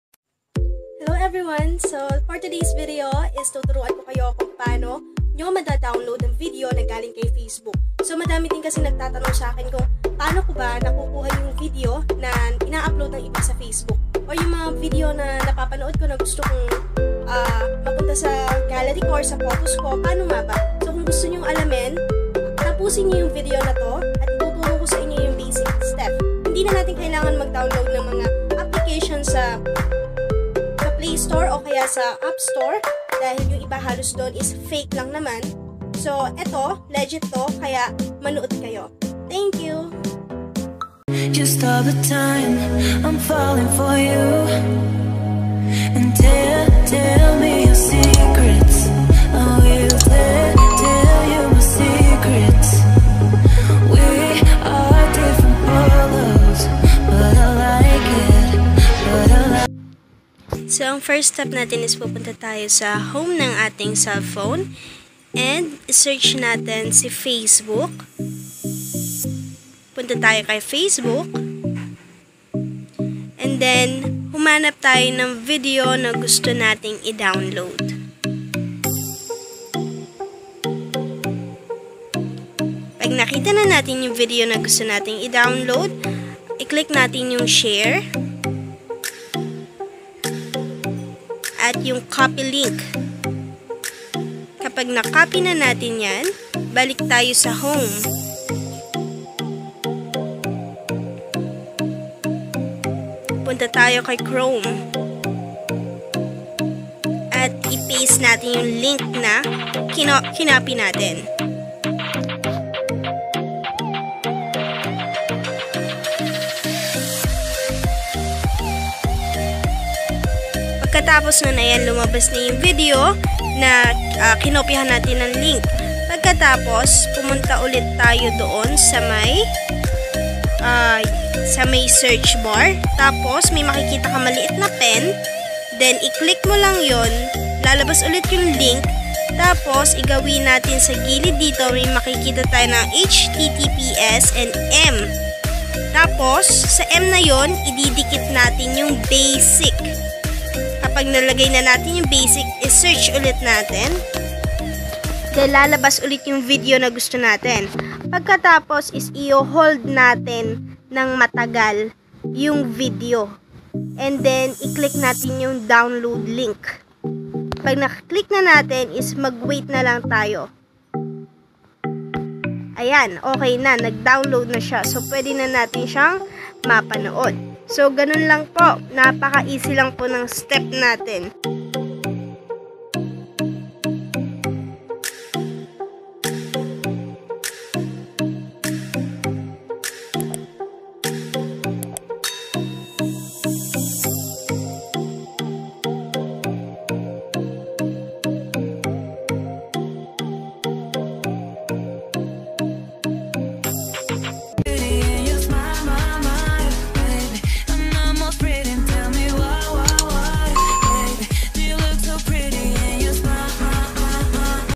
Paano mag Download ng video sa Facebook? | STEP BY STEP
For Today's Video ay ituturo ko sa inyo ang Basic step kung paano natin mado-download ang Video na galing sa Facebook at masave sa Gallery ! ☺️

If you have further questions or suggestion.